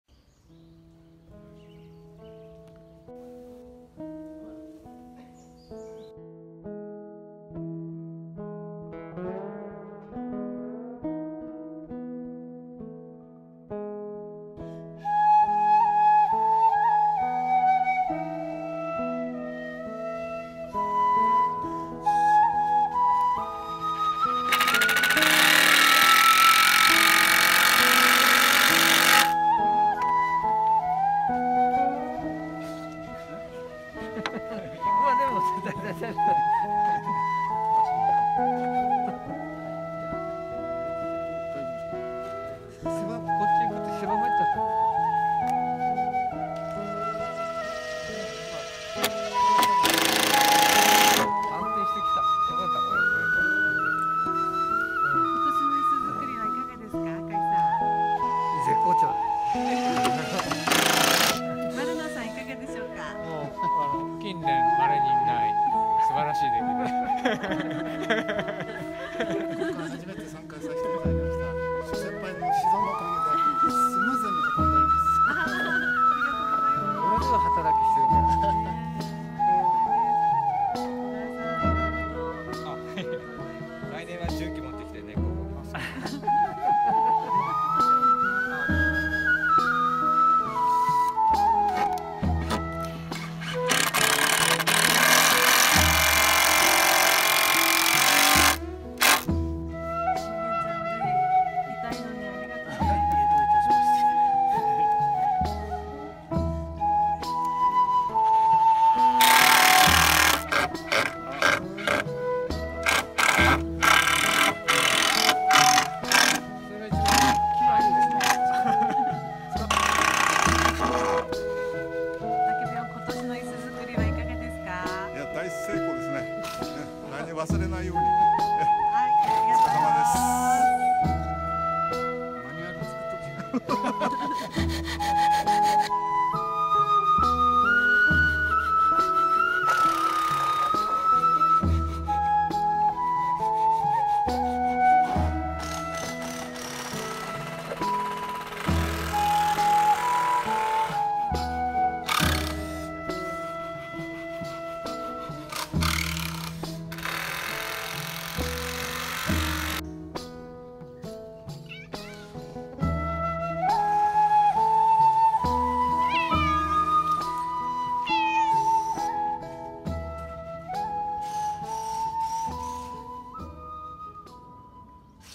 竹林コンサート椅子つくり
2019/4/21
武ぴょん、丸野さん、関野くん、小川さん、木村さん、武田くん
舞台、客席つくりに集まってくれました。
おかげさまで今年も安全安心の出来栄えとなりました。